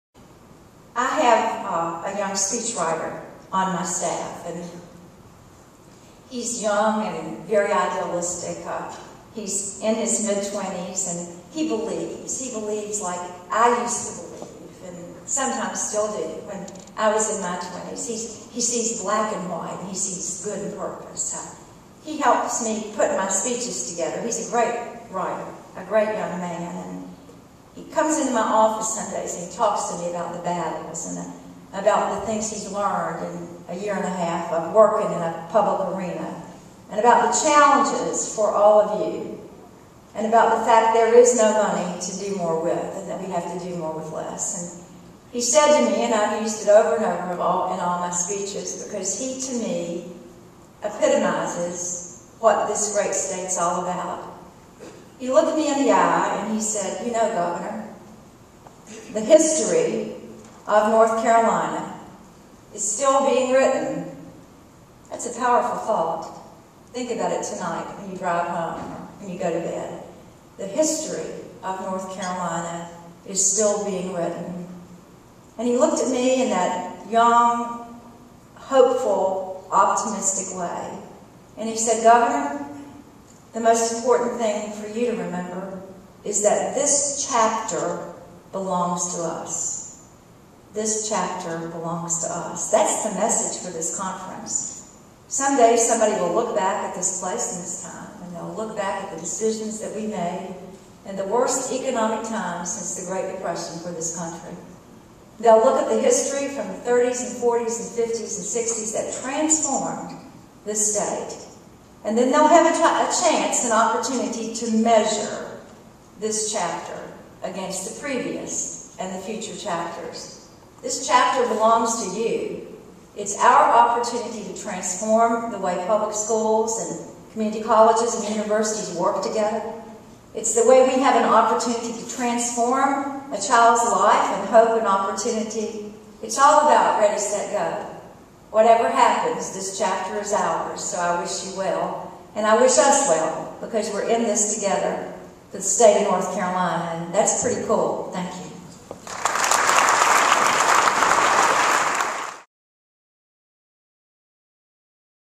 Video: Gov. Bev Perdue at "Many Voices, One Goal" conference (Part 3)
North Carolina Governor Bev Perdue offers a keynote address at the 2011 Many Voices, One Goal: Every North Carolina Child Graduates Ready conference. (Part 1 of 3)